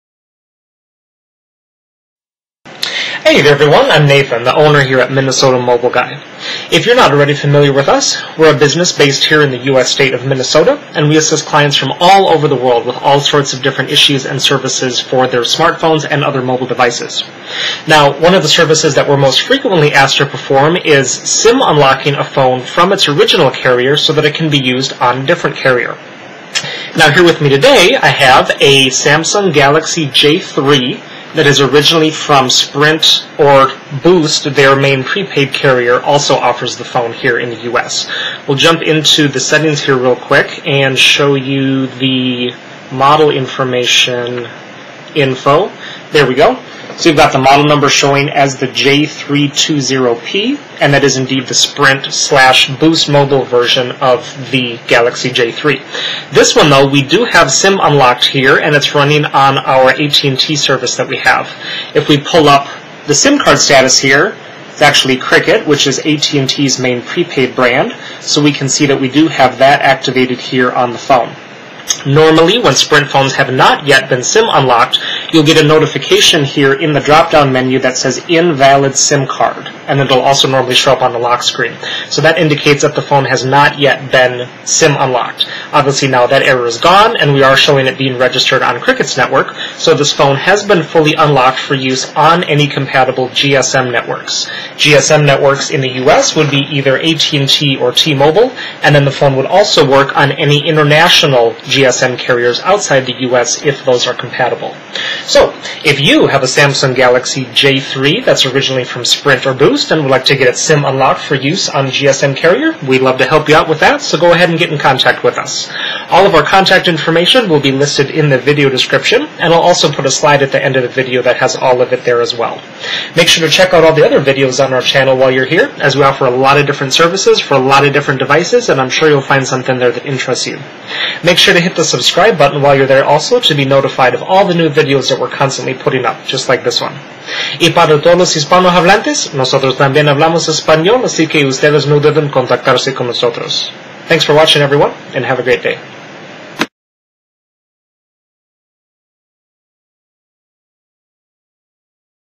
SIM Unlock Sprint / Boost / Virgin Samsung Galaxy J3 For Use On GSM Carriers!
Video showing a Sprint/Boost Mobile/Virgin Mobile Samsung Galaxy J3 (SM-J320P) which has been SIM unlocked for any GSM carrier - even US carriers!